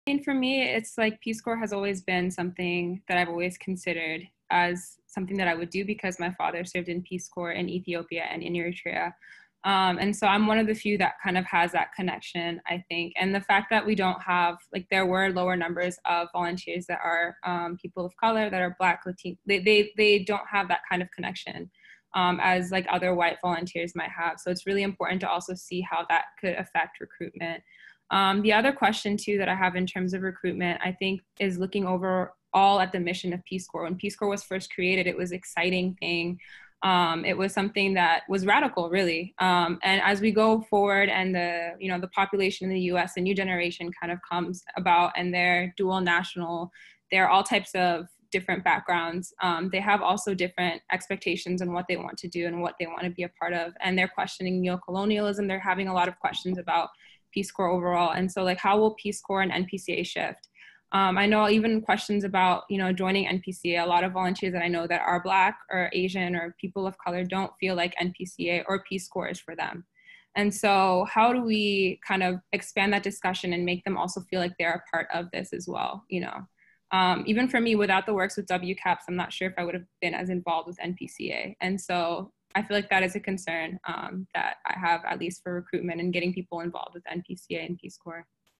Reimagining the Future of the Peace Corps: Marieme Foote — How Will Peace Corps and NPCA Shift?
On July 18, 2020, National Peace Corps Association hosted our global ideas summit: Peace Corps Connect to the Future. Returned Volunteer Marieme Foote assembled a panel of evacuated Volunteers — and one with a perspective from service in the ’60s — to discuss their experiences and ask how we can transform this moment in Peace Corps history.

Marieme Foote, Evacuated RPCV | Benin 2018–20
Rok Locksley, Evacuated RPCV | Philippines 2018–20
Juana Bordas | RPCV Chile 1966–68
Moderator: Glenn Blumhorst, President & CEO, NPCA | RPCV Guatemala 1988–91